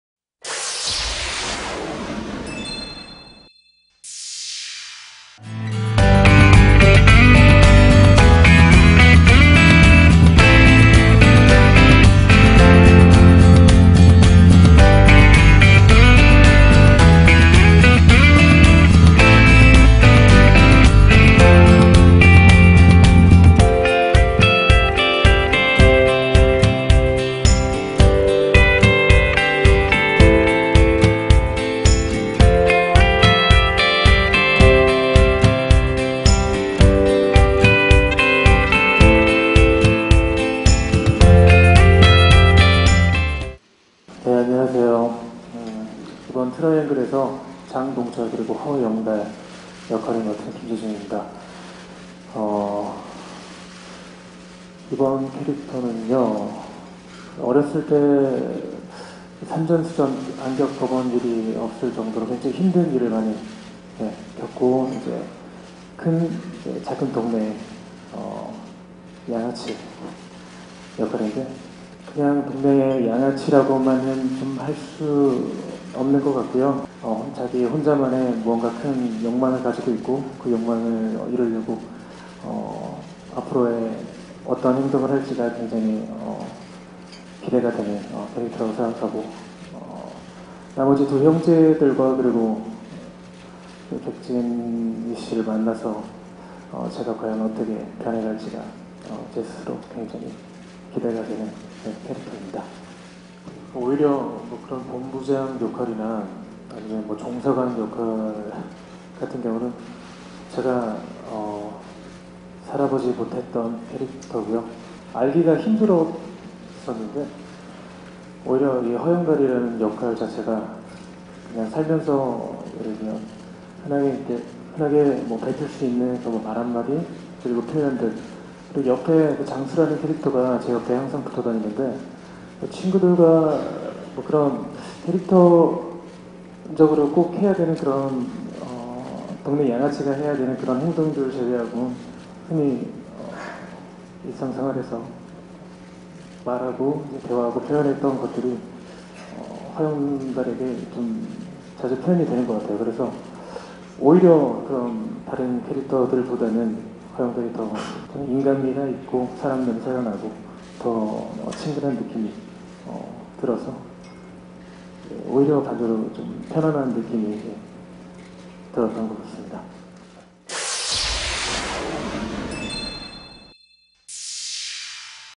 [tvdaily] ★KimJaeJoong★Drama 'Triangle' press conference
Drama 'Triangle' press conference_"General Manager role is more easy than a bad ass role"

[티브이데일리 강효진 기자] 김재중이 양아치 허영달 캐릭터를 맡게 된 소감을 전했다.

MBC 새 월화드라마 '트라이앵글'(극본 최완규, 연출 유철용)의 제작발표회가 30일 오후 3시 서울 임페리얼 팰리스 호텔에서 제작진 및 주연배우 이범수 김재중 임시완 오연수 백진희 등이 참석한 가운데 진행됐다.

이날 김재중은 "오히려 본부장이나 종사관 같은 경우는 제가 살아보지 못했던 캐릭터다. 잘 알기가 힘들었었는데 이 허영달이라는 역할 자체가 살면서 흔하게 접할 수 있는 표현들이 있다"고 운을 뗐다.

그는 "캐릭터적으로 꼭 해야하는 동네 양아치의 행동 말고는 허영달이라는 인물에는 제가 일상생활에서 평소에 말하고 표현했던 부분들이 담기는 것 같다. 오히려 다른 캐릭터보다는 인간미가 있고 사람냄새가 나고 친근한 느낌이 들어서 반대로 편안한 느낌이 들었던 것 같다"고 전했다.

이어 "이범수와 두 번째 호흡을 맞추게 된 소감"을 묻는 질문을 받은 그는 "존경하는 선배님이다. 같은 작품에서 두 번씩이나 호흡할 수 있어서 굉장히 설렌다. 제가 옆에서 선배님 하시는 것들을 많이 챙겨보면서 열심히 노력해야겠다고 생각하고 있다"고 덧붙여 눈길을 끌었다.

김재중은 이번 작품에서 삼형제의 둘째 장동철이자 카지노판에서 쓰레기처럼 살아가는 강원도 대표 양아치 허영달 역을 맡았다.

'트라이앵글'은 부모님을 잃고 뿔뿔이 흩어져 20년이 넘는 세월 동안 서로의 존재를 모르고 각기 다른 환경에서 자라온 삼형제의 이야기를 담는다. 이들이 핏줄의 운명으로 얽혀 그 인연을 이어가면서 벌어지는 인생과 사랑을 다루며 세상이 변해도 사랑의 힘은 영원하다는 고전적 주제를 새로운 작법으로 풀어나갈 예정이다. 다음달 5일 밤 10시 '기황후' 후속으로 첫 방송 된다.